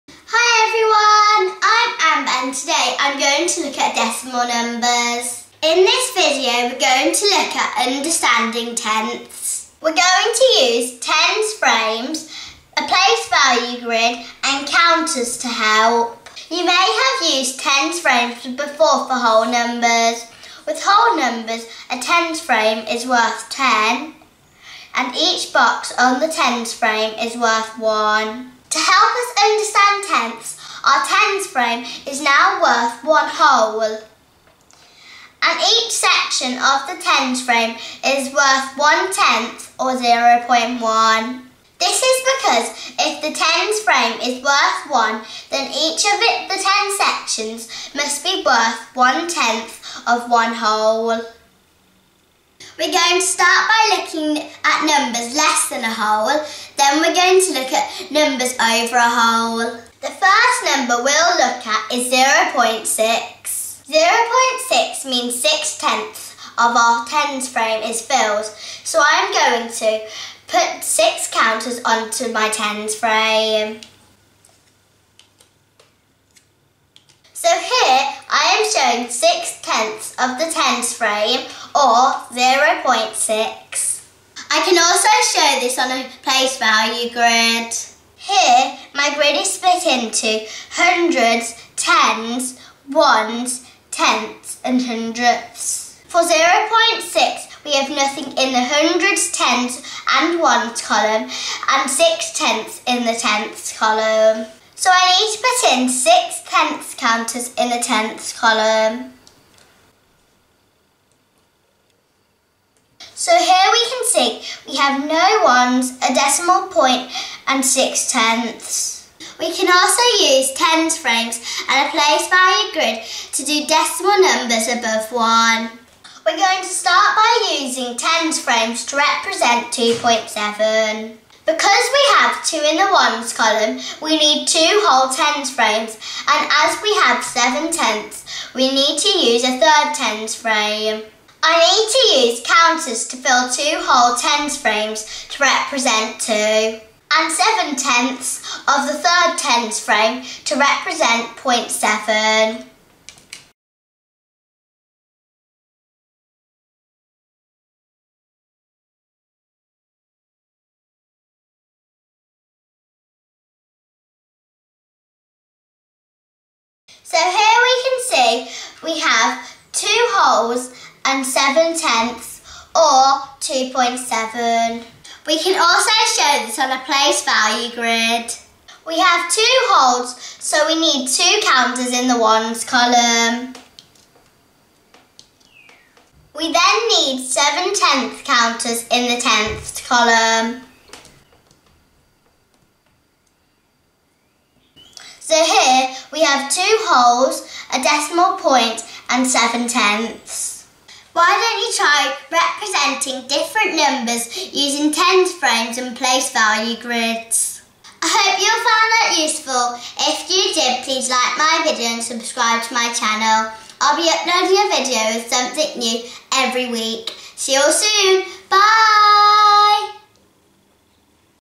Decimal numbers - tenths
In this video, we investigate decimal numbers, focusing on tenths. We use tens frames, a place value grid and counters to help children make sense of decimals.